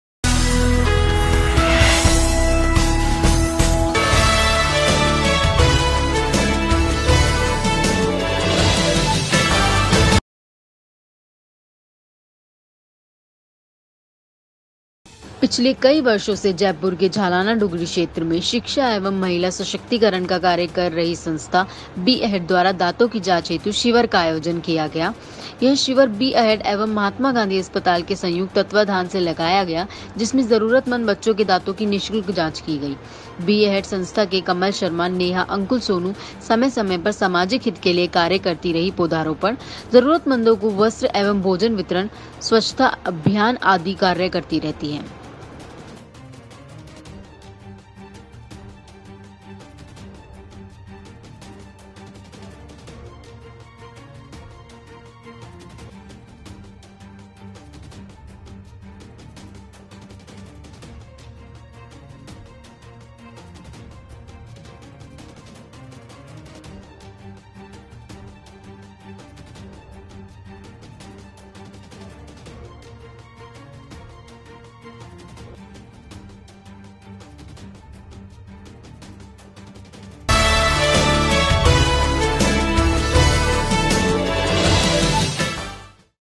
बी अहेड' द्वारा दाँतों की जाँच हेतु शिविर का आयोजन किया गया-Today24
पिछले कई वर्षों से जयपुर के झालाना डूँगरी क्षेत्र में शिक्षा एवं महिला सशक्तिकरण का कार्य कर रही संस्था 'बी अहेड' द्वारा दाँतों की जाँच हेतु शिविर का आयोजन किया गया। यह शिविर 'बी अहेड' एवं 'महात्मा गांधी अस्पताल' के संयुक्त तत्वावधान से लगाया गया जिसमें जरूरतमंद बच्चों के दाँतो की निःशुल्क जाँच की गई। बी अहेड' संस्था के कमल शर्मा. नेहा.अनुकुल.सोनू .समय समय पर सामाजिक हित के लिए कार्य करती रही पौधरोपण, जरूरतमंदो को वस्त्र एंव भोजन वितरण, स्वच्छता अभियान आदि कार्य करती रहती है। संवादाता विजयभवानी

T.V. News

Application.

views!!!

News."